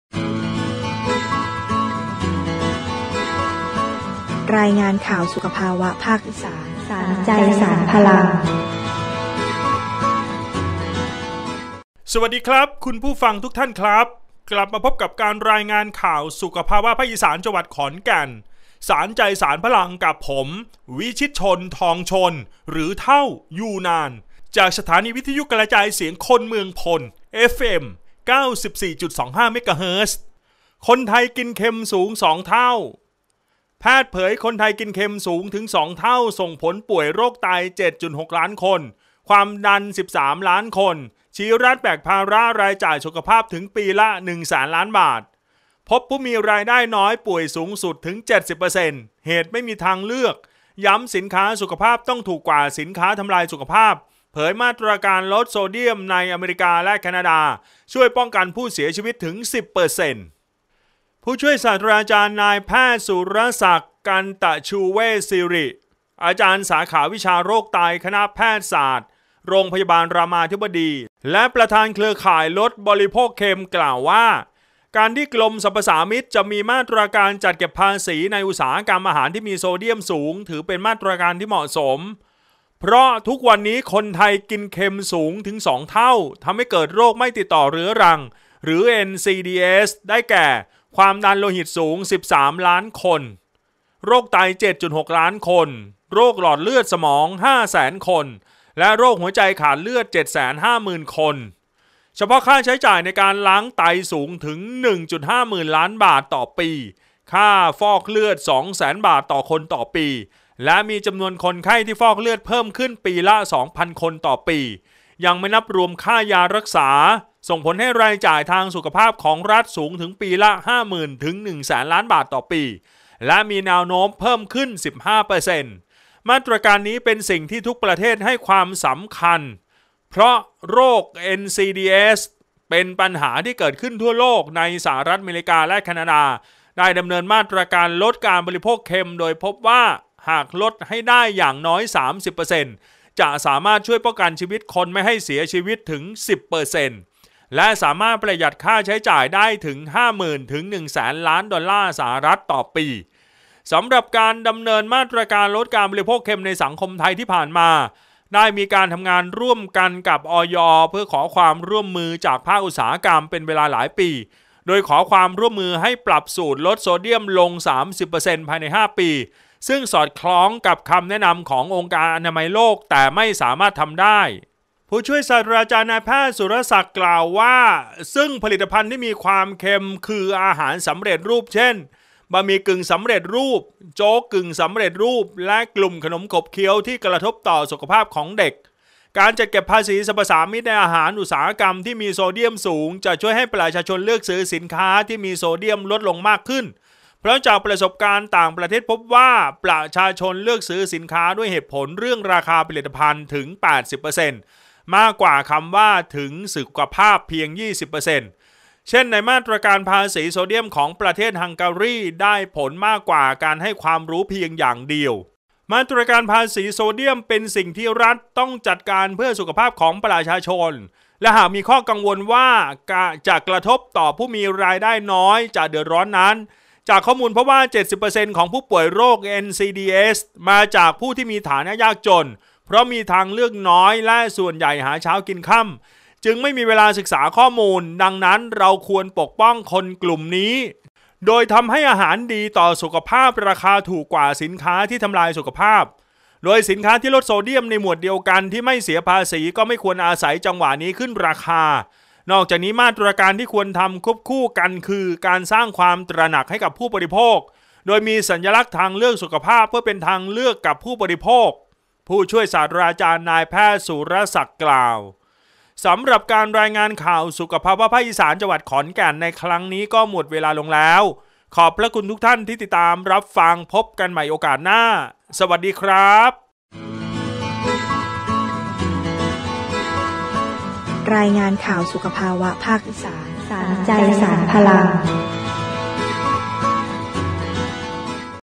รายงานข่าว สานใจ สานพลัง ภาคอีสาน ตอน คนไทยกินเค็มสูง 2 เท่า
แพทย์เผยคนไทยกินเค็มสูงถึง 2 เท่า ส่งผลป่วยโรคไต 7.6 ล้านคน ความดัน 13 ล้านคน ชี้รัฐแบกภาระรายจ่ายสุขภาพถึงปีละ 1 แสนล้านบาท พบผู้มีรายได้น้อยป่วยสูงสุดถึง 70% เหตุไม่มีทางเลือก ย้ำสินค้าสุขภาพต้องถูกกว่าสินค้าทำลายสุขภาพ เผยมาตรการลดโซเดียมในอเมริกา-แคนาดา ช่วยป้องกันผู้เสียชีวิตถึง 10%

ผศ.นพ.สุรศักดิ์ กันตชูเวสศิริ อาจารย์สาขาวิชาโรคไต คณะแพทยศาสตร์โรงพยาบาลรามาธิบดีและประธานเครือข่ายลดบริโภคเค็ม กล่าวว่า การที่กรมสรรพสามิตจะมีมาตรการจัดเก็บภาษีในอุตสาหกรรมอาหารที่มีโซเดียมสูง ถือเป็นมาตรการที่เหมาะสม เพราะทุกวันนี้คนไทยกินเค็มสูงถึง 2 เท่า ทำให้เกิดโรคไม่ติดต่อเรื่องรัง หรือNCDs ได้แก่ ความดันโลหิตสูง 13 ล้านคน โรคไต 7.6 ล้านคน โรคหลอดเลือดสมอง 5 แสนคน และโรคหัวใจขาดเลือด750,000 คน เฉพาะค่าใช้จ่ายในการล้างไตสูงถึง 1.5 หมื่นล้านบาท/ปี ค่าฟอกเลือด 2 แสนบาท/คน/ปี และมีจำนวนคนไข้ที่ฟอกเลือดเพิ่มขึ้นปีละ 2,000 คน/ปี ยังไม่นับรวมค่ายารักษา ส่งผลให้รายจ่ายทางสุขภาพของรัฐสูงถึงปีละ 50,000-100,000 ล้านบาท/ปี และมีแนวโน้มเพิ่มขึ้น 15% มาตรการนี้เป็นสิ่งที่ทุกประเทศให้ความสำคัญเพราะโรค NCDs เป็นปัญหาที่เกิดขึ้นทั่วโลก ในสหรัฐอเมริกาและแคนาดาได้ดำเนินมาตรการลดการบริโภคเค็ม โดยพบว่าหากลดให้ได้อย่างน้อย 30% จะสามารถช่วยป้องกันชีวิตคนไม่ให้เสียชีวิตถึง 10% และสามารถประหยัดค่าใช้จ่ายได้ถึง 50,000-100,000 ล้านดอลลาร์/ปี สำหรับการดำเนินมาตรการลดการบริโภคเค็มในสังคมไทย ที่ผ่านมาได้มีการทำงานร่วมกับ อย.เพื่อขอความร่วมมือจากภาคอุตสาหกรรมเป็นเวลาหลายปี โดยขอความร่วมมือให้ปรับสูตรลดโซเดียมลง 30% ภายใน 5 ปี ซึ่งสอดคล้องกับคำแนะนำขององค์การอนามัยโลกแต่ไม่สามารถทำได้

ผศ.นพ.สุรศักดิ์ กล่าวว่า ซึ่งผลิตภัณฑ์ที่มีความเค็มสูงคือ อาหารสำเร็จรูป เช่น บะหมี่กึ่งสำเร็จรูป โจ๊กกึ่งสำเร็จรูป และกลุ่มขนมขบเคี้ยวที่กระทบต่อสุขภาพของเด็ก การจัดเก็บภาษีสรรพสามิตในอาหารอุตสาหกรรมที่มีโซเดียมสูงจะช่วยให้ประชาชนเลือกซื้อสินค้าที่มีโซเดียมลดลงมากขึ้น เพราะจากประสบการณ์ต่างประเทศพบว่า ประชาชนเลือกซื้อสินค้าด้วยเหตุผลเรื่องราคาผลิตภัณฑ์ถึง 80% มากกว่าคำถึงถึงสุขภาพเพียง 20% เช่นในมาตรการภาษีโซเดียมของประเทศฮังการีที่ได้ผลมากกว่าการให้ความรู้เพียงอย่างเดียว

“มาตรการภาษีโซเดียมเป็นสิ่งที่รัฐต้องจัดการเพื่อสุขภาพของประชาชน และหากมีข้อกังวลว่าจะกระทบต่อผู้มีรายได้น้อยจะเดือดร้อนนั้น จากข้อมูลพบว่า 70% ของผู้ป่วยโรคNCDs มาจากผู้ที่มีฐานะยากจน เพราะมีทางเลือกน้อยและส่วนใหญ่หาเช้ากินค่ำจึงไม่มีเวลาศึกษาข้อมูล ดังนั้นเราควรปกป้องกลุ่มคนเหล่านี้โดยทำให้อาหารดีต่อสุขภาพราคาถูกกว่าสินค้าที่ทำลายสุขภาพ โดยสินค้าที่ลดโซเดียมในหมวดเดียวกันที่ไม่เสียภาษีก็ไม่ควรอาศัยจังหวะนี้ขึ้นราคา นอกจากนี้มาตรการที่ควรทำควบคู่กันคือ การสร้างความตระหนักให้กับผู้บริโภคโดยมีสัญลักษณ์ทางเลือกสุขภาพเพื่อเป็นทางเลือกกับผู้บริโภค” ผศ.นพ.สุรศักดิ์ กล่าว